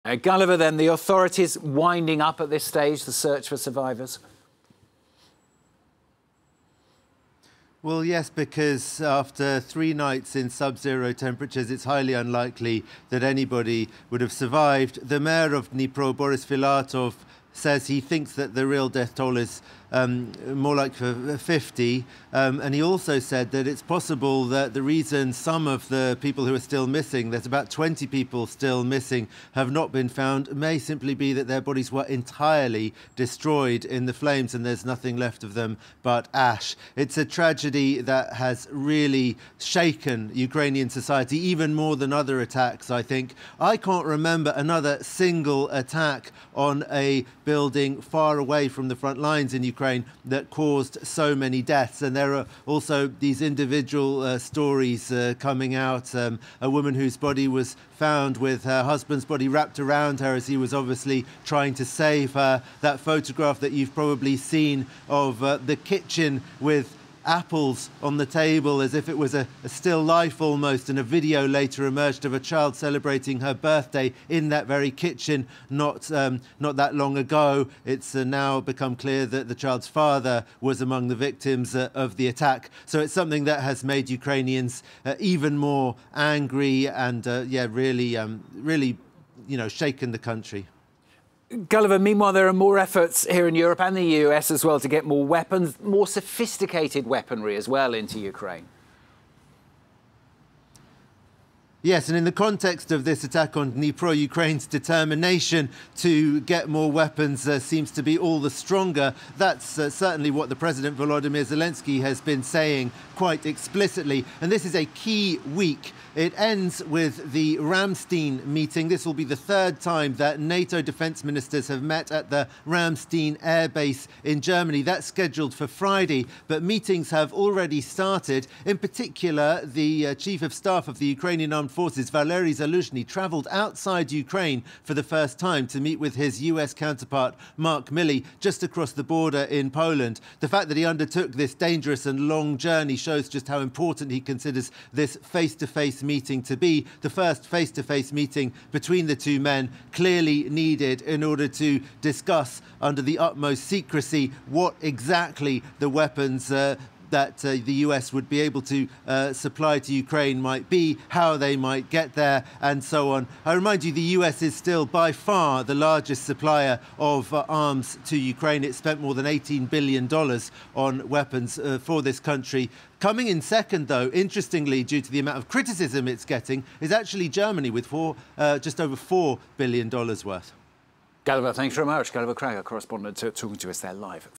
Shock at Dnipro civilian deaths spurs calls for more Western weapons for Ukraine • FRANCE 24
Rescuers have called off the search for victims of the Russian missile strike on an apartment building that killed at least 45 people in the Ukrainian city of Dnipro, with 20 people still missing and funerals being held in the grief-stricken community. The grim tally comes as Ukraine has moved a step closer to winning approval for German-made modern battle tanks and has secured a pledge of more Patriot defence missiles from the West. FRANCE 24's Gulliver Cragg reports from the capital Kyiv.